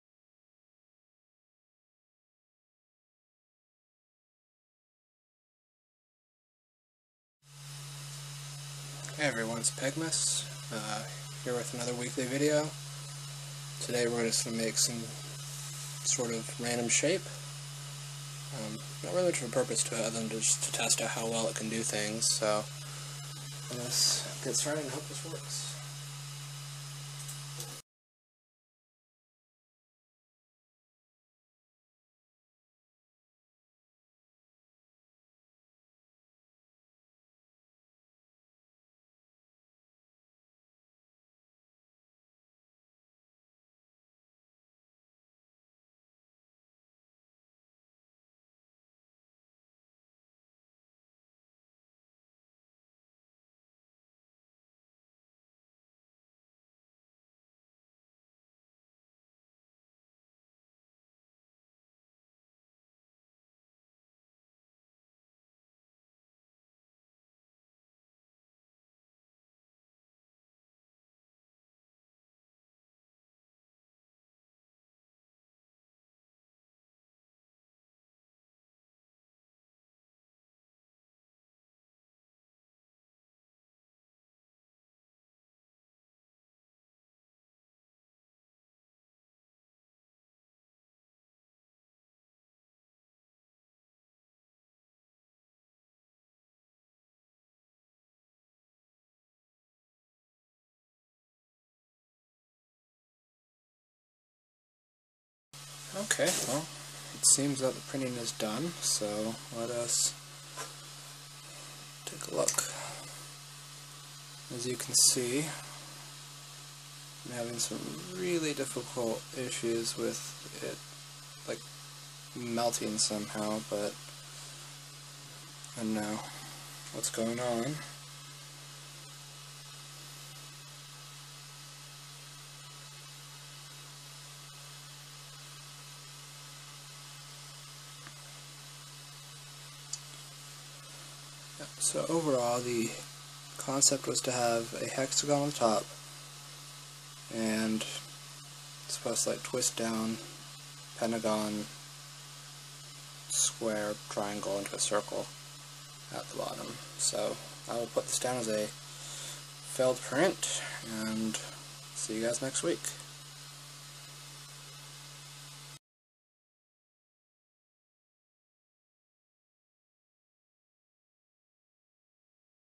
3d printed art. Week 4 with my solidoodle printer.
The fourth week with my printer. Tried making an art piece but something went just a lot wrong, But the technology is new, and i refuse to give up.